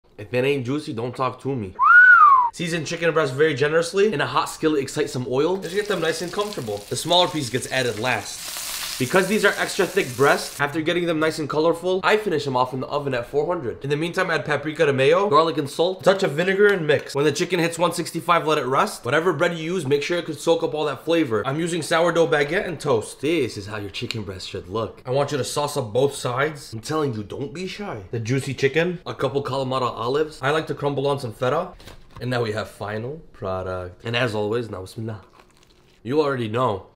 A Juicy Grilled Chicken Sandwich | The Golden Balance
2 lbs Chicken Breast ( 4 pieces)
Seasoning:
2 tsp salt
2 tsp smoked paprika
2 tsp garlic powder
1 tsp black pepper

1 large sour dough baguette or what ever bread you prefer

1 cup mayo
1 Tbsp paprika
1 tsp salt
1 tsp garlic powder
1 Tbsp white vinegar

Finish with handful of kalamata olives and feta cheese